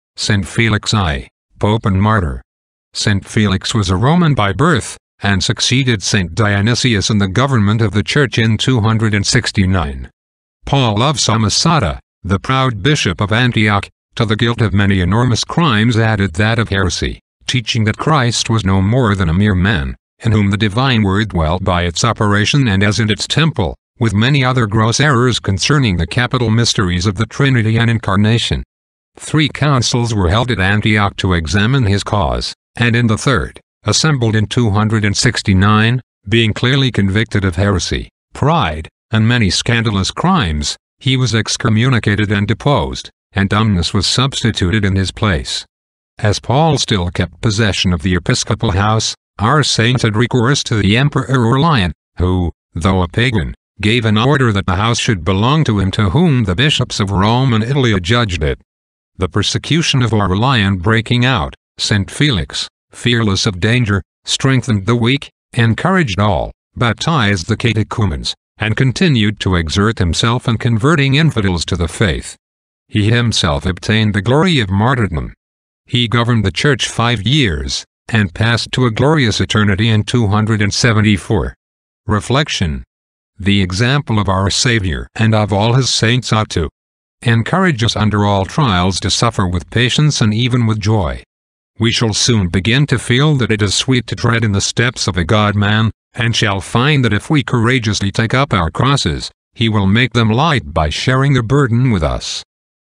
May 30 - Saint Felix - Pope - Martyr - 0274 - Rome
From the Little Pictorial Lives of the Saints with reflections for every day in the year, May 30th.